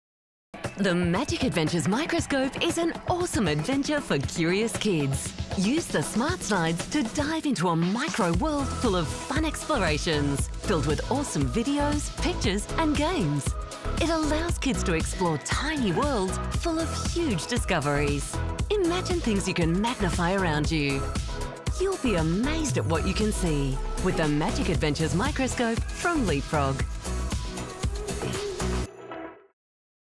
Magic Adventures Microscope | LeapFrog | TV Commercial | 0:30
Explore tiny worlds full of huge discoveries with the Magic Adventures Microscope. Young biologists can zoom in on flowers, animals, food, minerals and more using eight double-sided smart slides that activate amazing BBC videos and images.